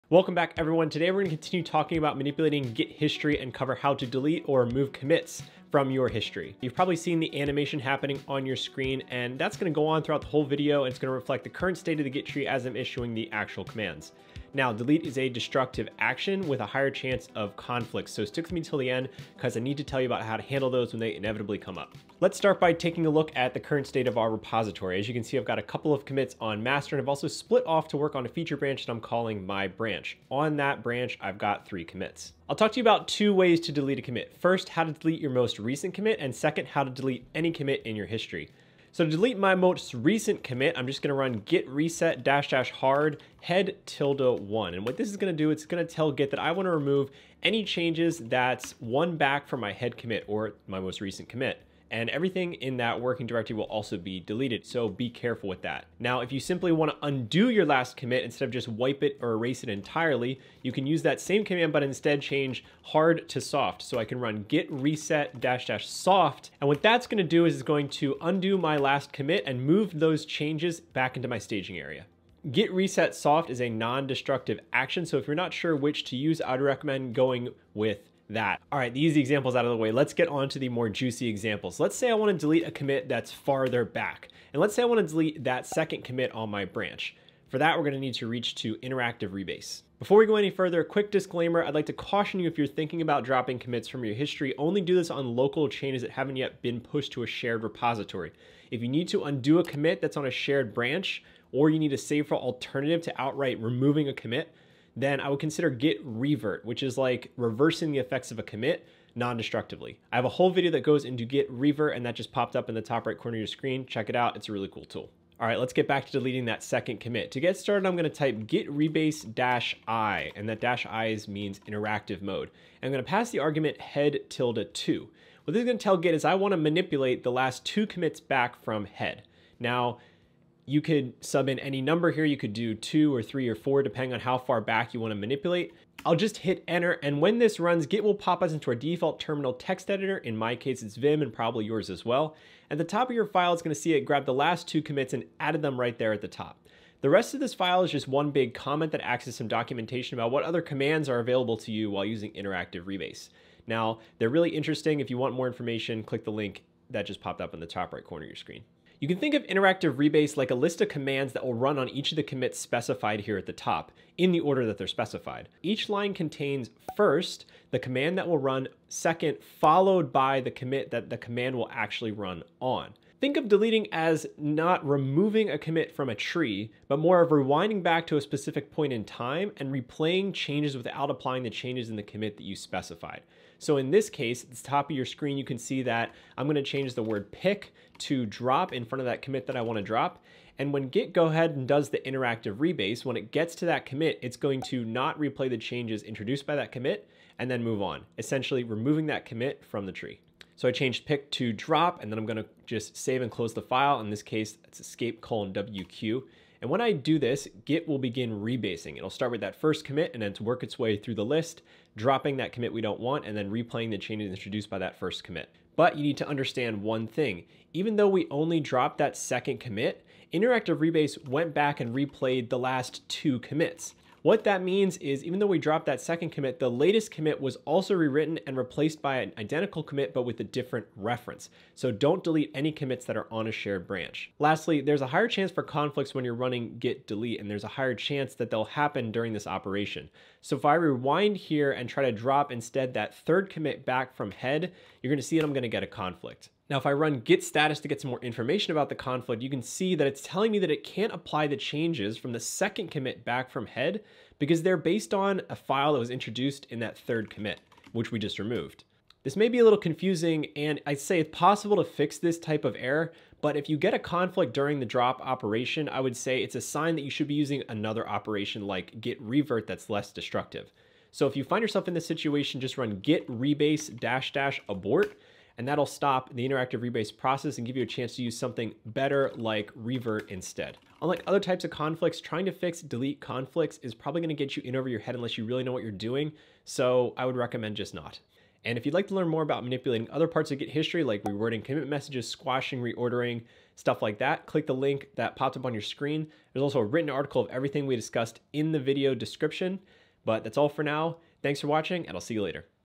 Delete Git Commits Tutorial
CHAPTERS
0:00 Intro
0:35 How to delete your most recent git commit
1:20 How to delete any git commit
2:16 Using interactive rebase to delete commits
3:54 How to resolve git delete merge conflicts